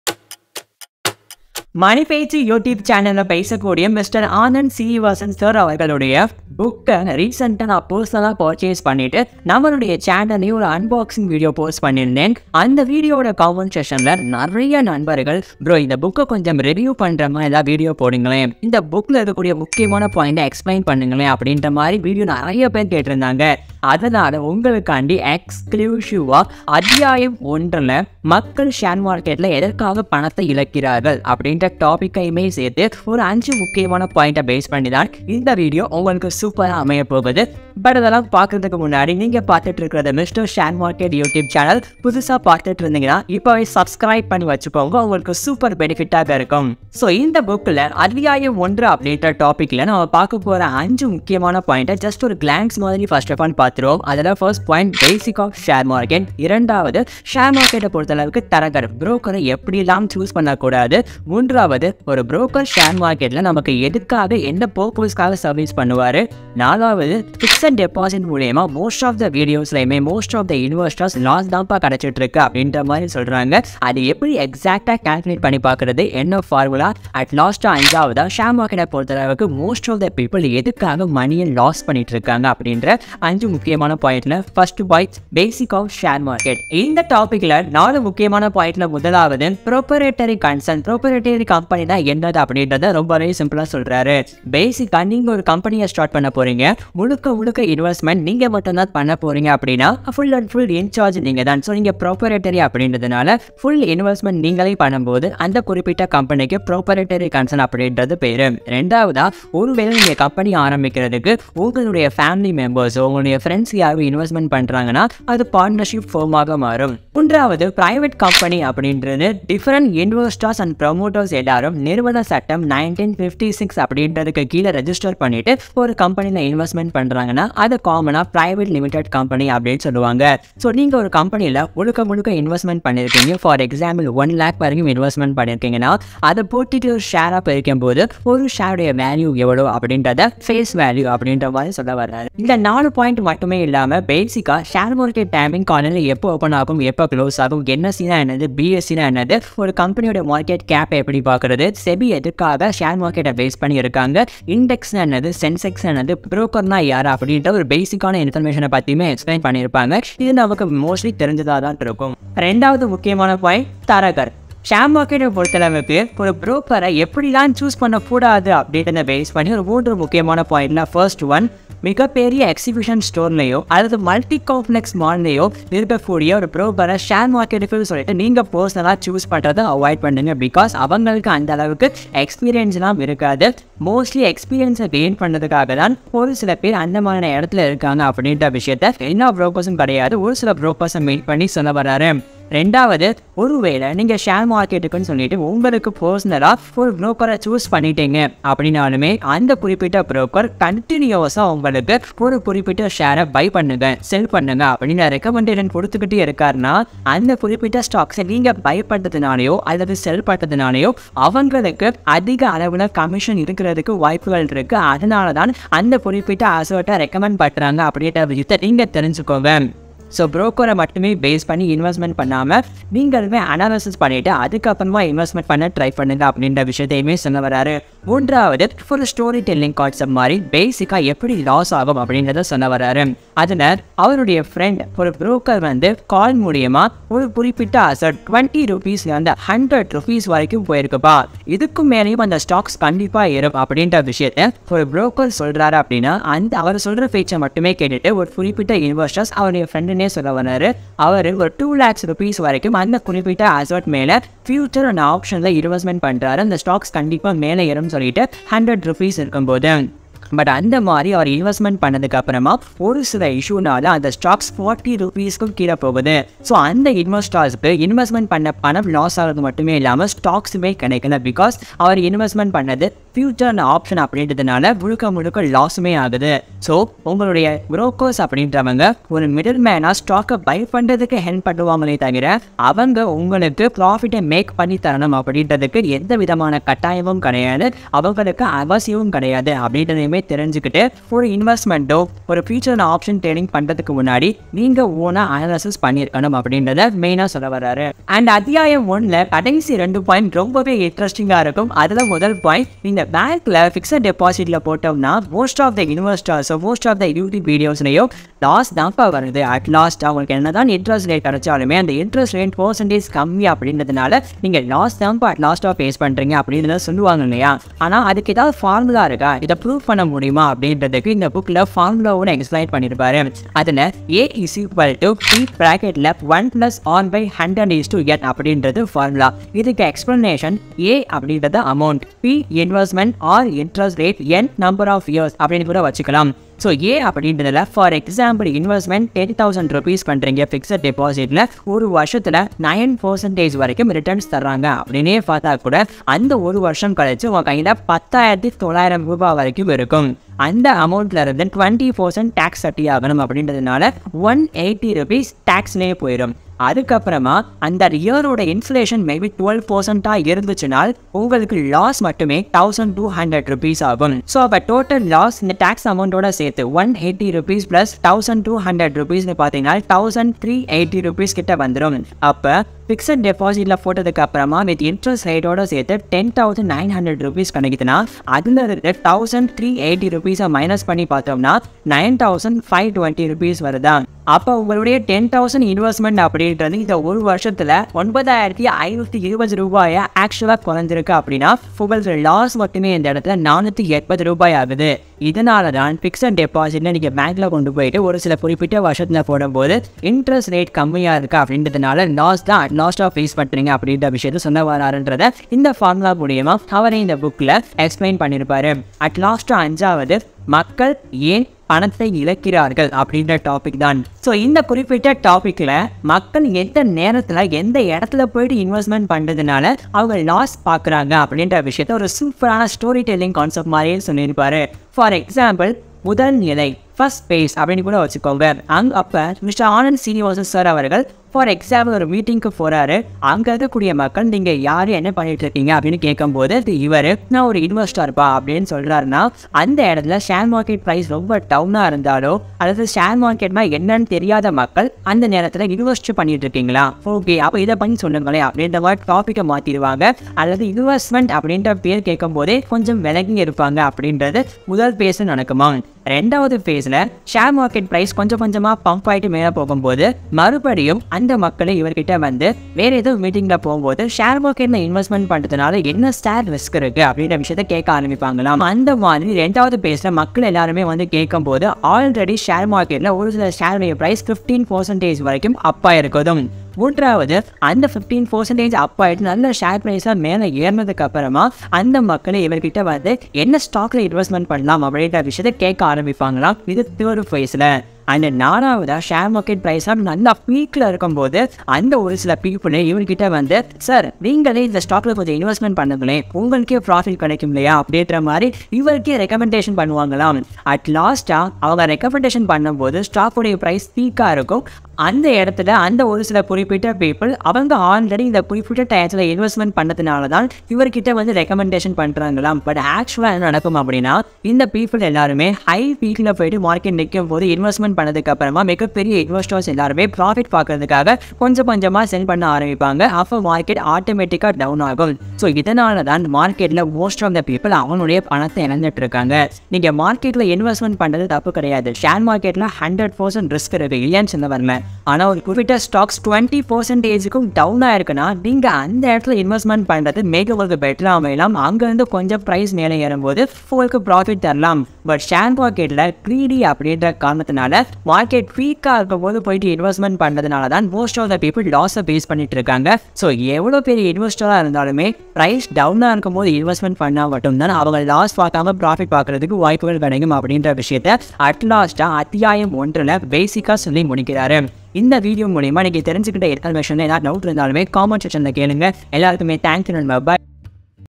Money Pechu - Mr.Anand Srinivasan 😍( Stocks Book about 🤑 Money)
Money Pechu - Mr.Anand Srinivasan 😍( Stocks Book about 🤑 Money) | Tamil | Mr.Share Market

In this video I explained about Mr.Share Srinivasan's book called investors ordinary stock more profit review for educational Purpose about why people lose money then if you want to purchase book means kindly visit money pechu youtube channel.